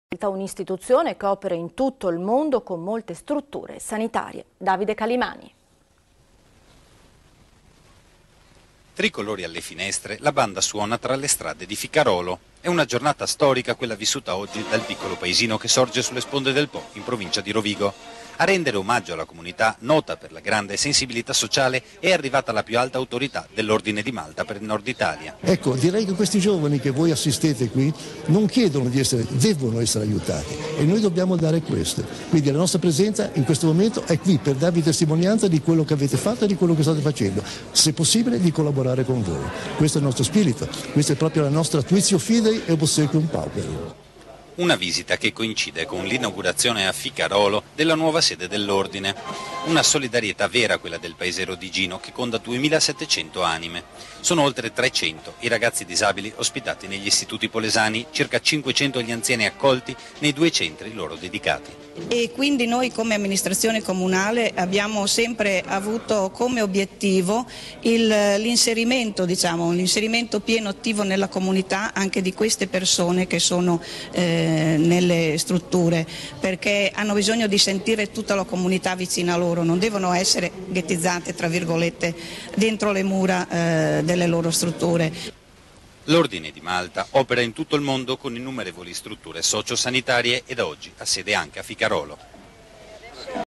Banda di Lendinara a Ficarolo per L'Ordine di Malta
in occasione del 2 Giugno (giorno dell'unità d'italia) si è anche celebrata l'inaugurazione della sede di Ficarolo dell'Ordine di Malta